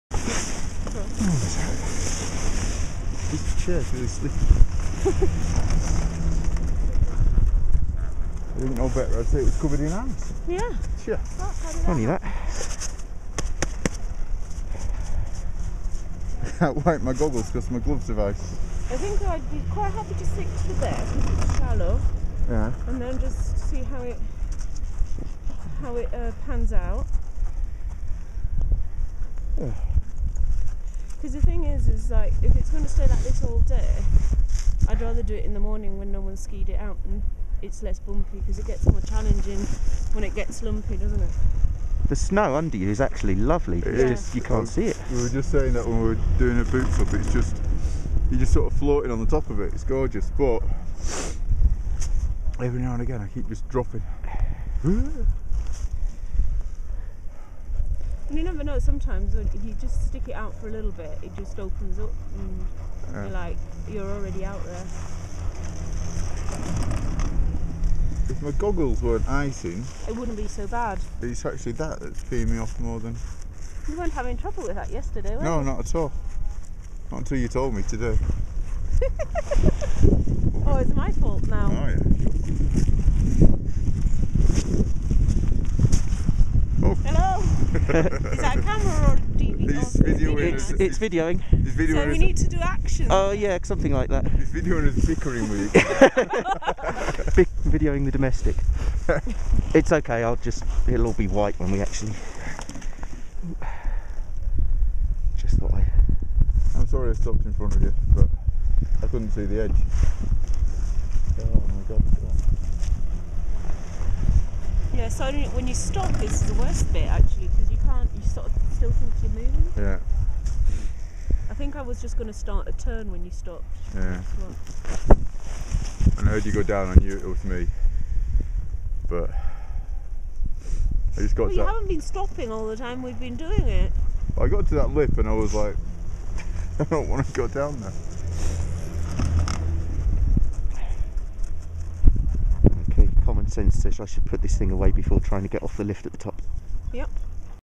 Big White Chairlift Ride
Part of a trip up the Black Forest chair at Big White BC, Jan 2011